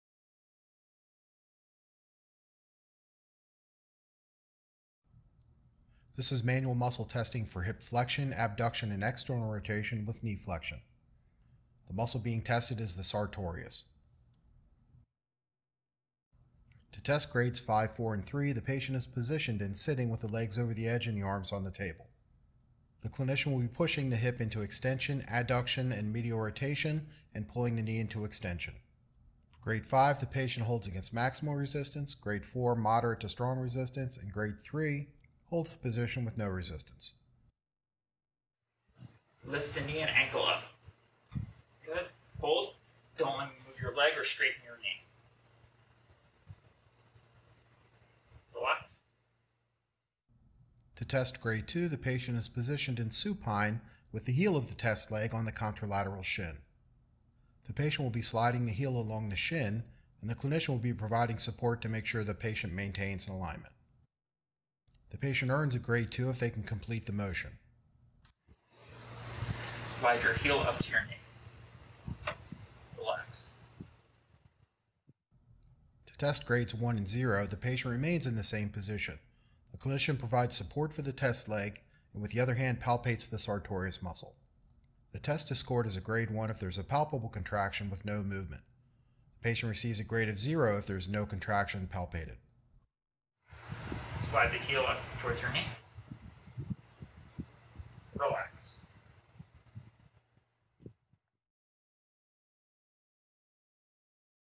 Manual Manual Test for the Sartorius Muscle
Learn the proper technique to perform a manual muscle test for the sartorius muscle of the hip/thigh.